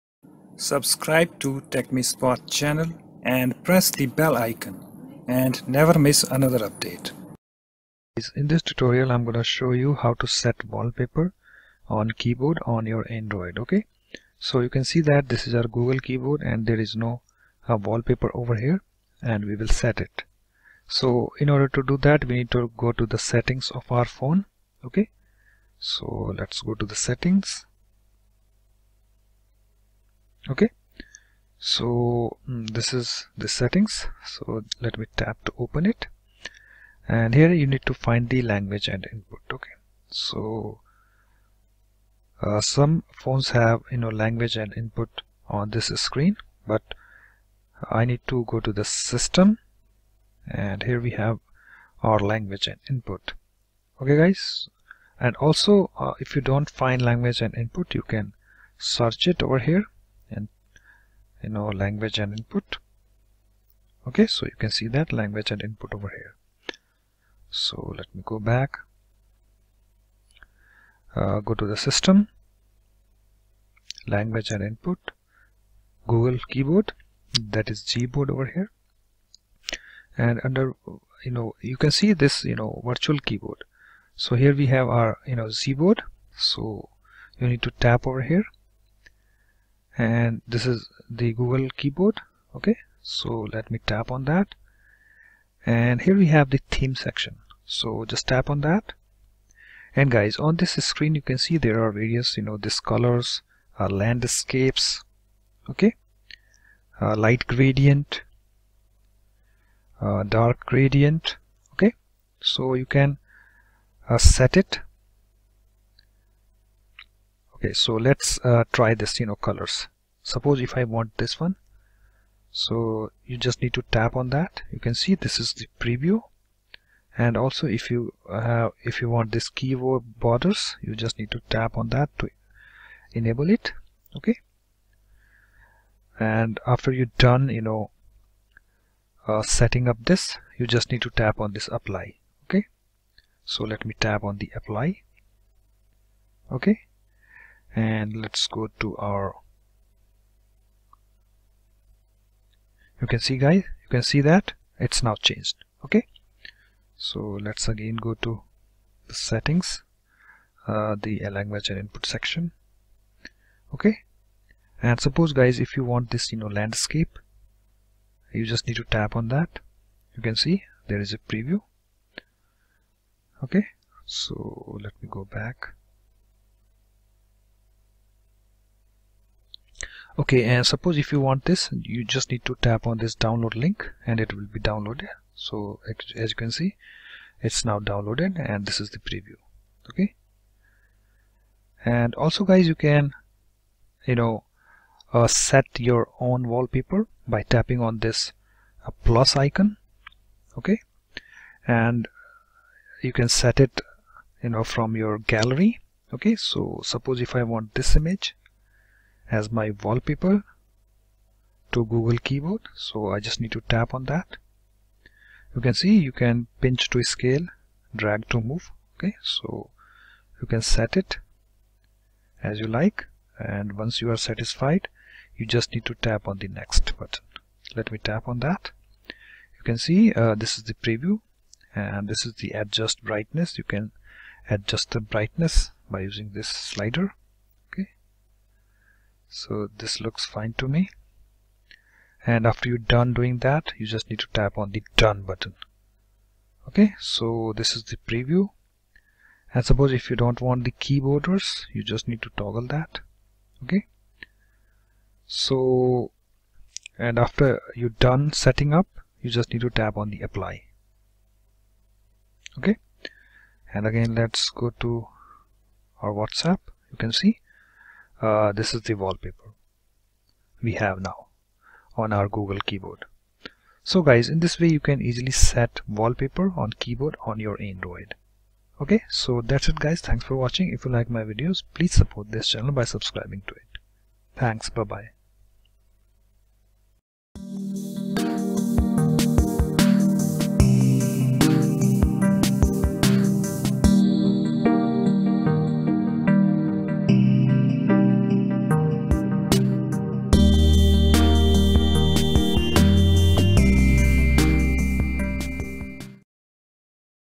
How to Set Wallpaper on Keyboard on your Android?
Step 1: Go to 'Settings' of your Phone.

Step 2: Go to 'Language and Input' section.

Step 3: Tap on Gboard, and then tap on Theme.

Step 4: Now, you can see various colors, gradient, light gradient. Just Tap on your choice, and the Tap on 'Apply'.

Step 5: Also, you can set your own wallpaper by Tapping on '+' icon at the top, and Select it from gallery.

It's done.